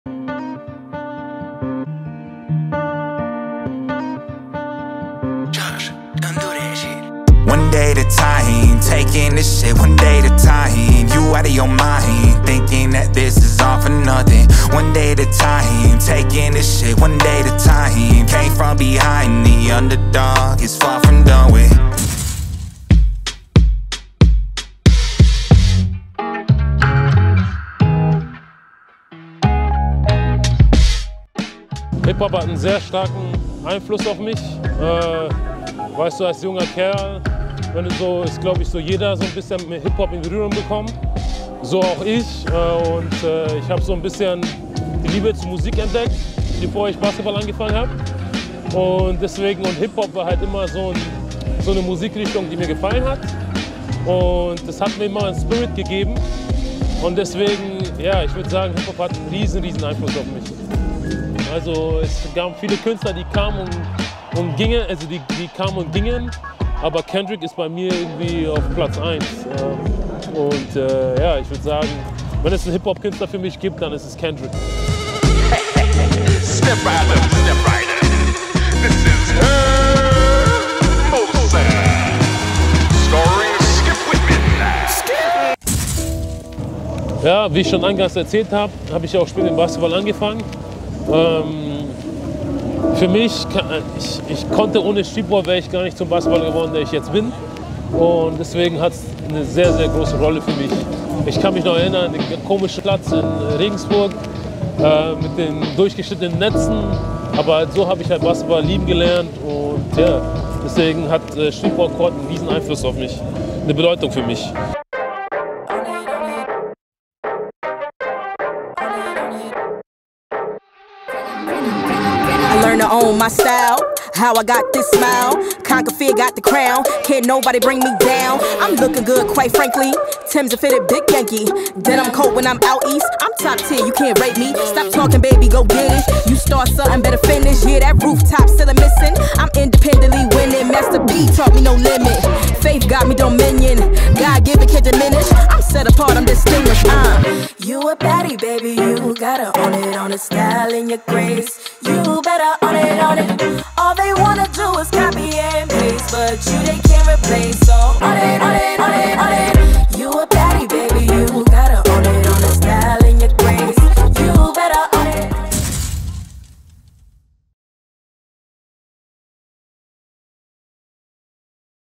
HIPHOP OPEN 🤝 MHP RIESEN Ludwigsburg: Yorman Polas Bartolo und Eddy Edigin auf den HipHop OPEN 23
HipHop x Basketball - passt wie Faust aufs Auge‼️

Es war uns eine Ehre nach achtjähriger HipHp Open-Pause mit einem Basketballcourt auf dem Cannstatter Wasen 2023 vertreten gewesen zu sein. Yorman Polas Bartolo und Eddy Edigin trafen im Artist Village auf Künstler wie OG Keemo, Digga D, Sido, CKay und viele mehr.  Außerdem performte auch das Liberty Dance Team der New York City Dance School auf unserem Court.